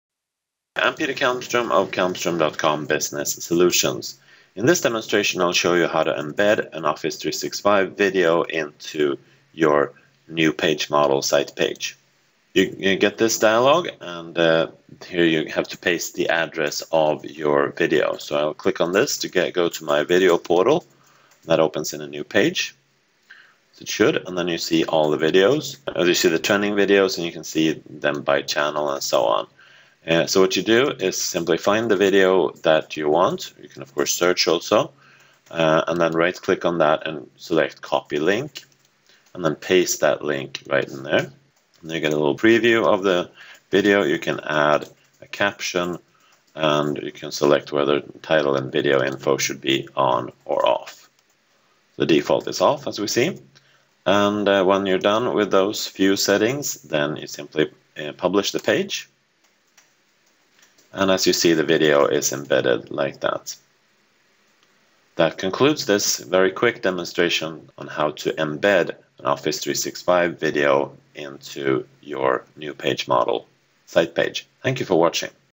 SharePoint Online New Page Model - Office 365 Video Web Part
Learn how to add an Office 365 video to a SharePoint Online new model